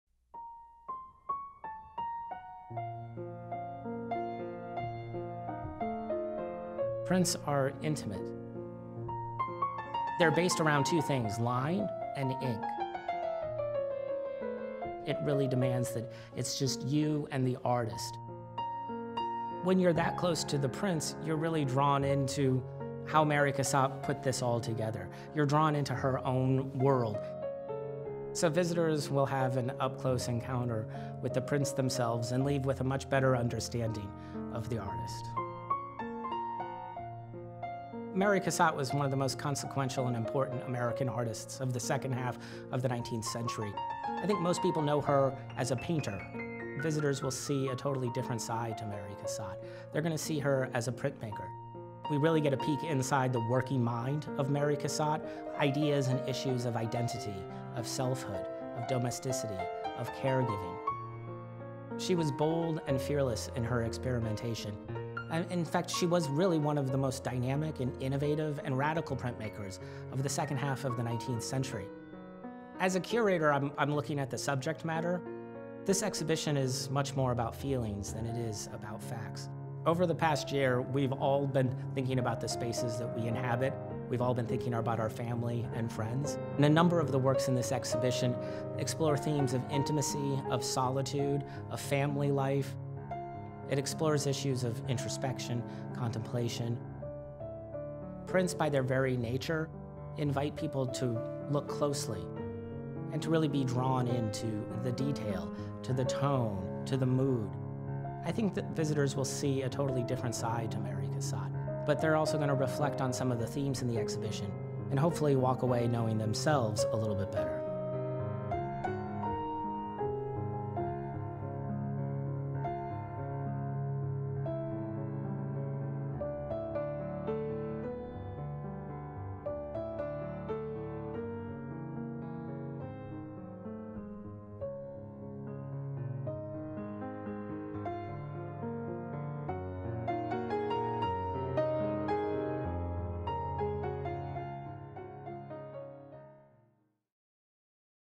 Inside Out: The Prints of Mary Cassatt
Mary Cassatt’s life hit a turning point in 1877 when the artist Edgar Degas invited her to exhibit with the Impressionists. Cassatt was overjoyed: “At last I could work with absolute independence.” Free from the rigid and tiresome conventions of the Paris Salon (the prevailing venue in which artists showed their work at the time), Cassatt began to pursue her own creative vision. This path led her to printmaking, a field altogether new to her. She quickly gained proficiency and became known as one of the most innovative printmakers of the nineteenth century. In 1879 she partnered with Degas and Camille Pissarro on a project to publish a journal devoted to the fine art of prints. Although the publication never came to fruition, the project drove Cassatt toward ever more radical experiments in printmaking. These revealed a different side of the American artist who had moved into the center of the French art world.

Curated by Justin McCann, Lunder Curator of Whistler Studies at the Colby Museum, and Shalini LeGall, Chief Curator, Susan Donnell and Harry W. Konkel Curator of European Art, and Director of Academic Engagement at the Portland Museum of Art, Inside Out: The Prints of Mary Cassatt examines Cassatt’s printmaking from the perspective of the artist herself. It focuses on intersections between her prints and her personal concerns with identity and selfhood, family and child care, creativity and making. Visitors will engage closely with an artist who used printmaking to capture and make sense of the people and the spaces nearest and dearest to her.

Inside Out: The Prints of Mary Cassatt will be on view June 17–November 1, 2021.